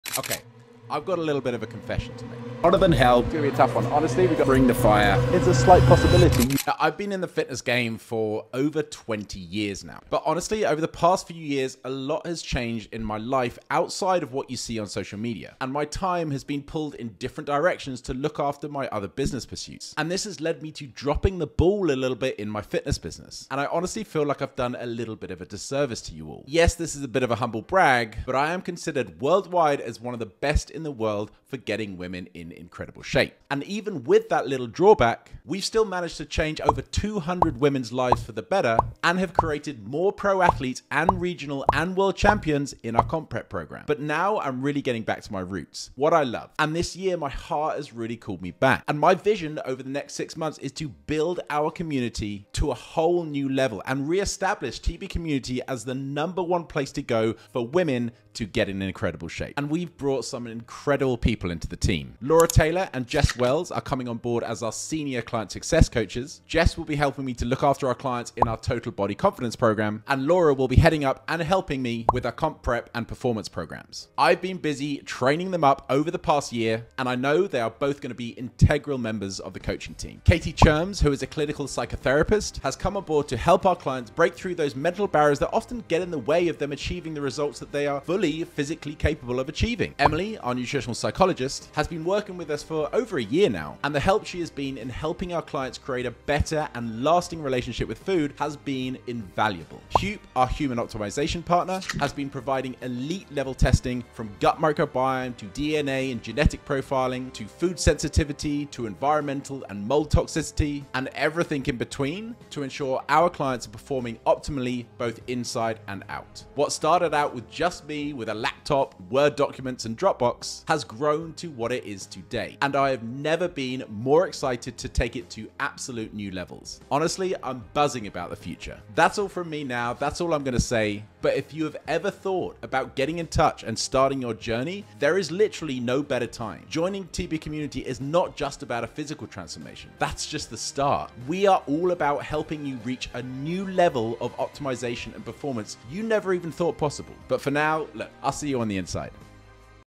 I've Got A Confession to Make
I’ve dropped the ball in the fitness game over the past few years… and I apologize for that.

BUT!! I’m back and ready to take the TB Community to a whole new level.

Even with that setback, we’ve still managed to change over 200 women’s lives and create pro athletes, regional, and world champions… but now I’m more fired up than ever to expand the team to meet the growing demand. 🙌

It’s come a long way since it was just me with my laptop and Dropbox.

I’ve had a great team behind me for the last few years, but I’m pushing the boundaries, expanding the team, and bringing them into the forefront.

Laura Taylor & Jess Wells will be our Senior Client Success Coaches, helping me with our Total Body Confidence, comp prep, and performance programs.

Katie Churms, our in-house Psychotherapist, helps our clients break through mental barriers holding them back from achieving the results they are more than capable of achieving.

Emelie, our Nutritional Psychologist and co-founder of @freely.project, has been helping our clients develop a lasting, healthy relationship with food.

Hupe, our partner, and a cutting-edge Human Optimization Platform, provides the most comprehensive testing, diagnostics, programming, and support for peak performance and a truly optimal lasting state.

We’re not just about getting you in shape—we’re focused on making you the absolute best version of yourself, mind and body.

Our doors are OPEN—whether you’re a woman in your 40s going through menopause or hormonal imbalances and struggling with weight loss, or a fitness athlete wanting that Pro Card or World Championship… we have you covered.

DM us the word “READY”—that’s ALL you have to do!!

Don’t waste another year feeling like someone you’re not proud of… Start looking after number 1 and I guarantee you will be a way better human to your loved ones when you do. 👊🏼